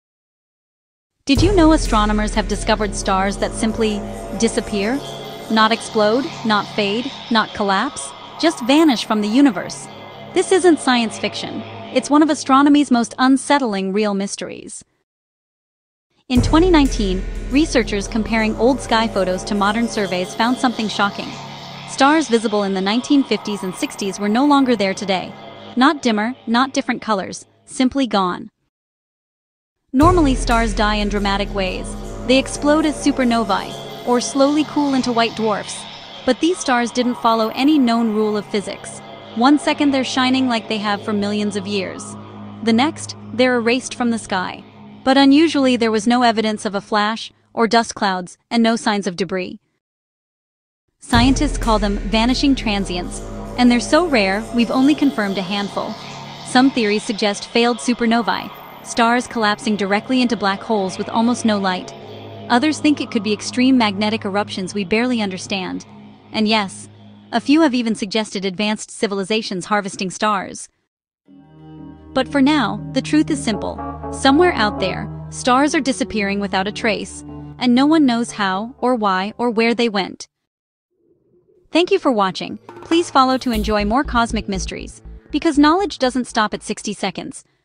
The Stars That Vanished Without a Trace
What happens when a star disappears without exploding, fading, or leaving anything behind?
Astronomers studying archival sky photos have discovered stars that were visible in the mid-1900s but are missing today — with no known explanation.

These “vanishing stars” break every rule we understand about stellar physics.
Some may be failed supernovae.
Some may be extreme magnetic events.
And some might be something we haven’t even imagined.

Here’s the mystery scientists still can’t solve.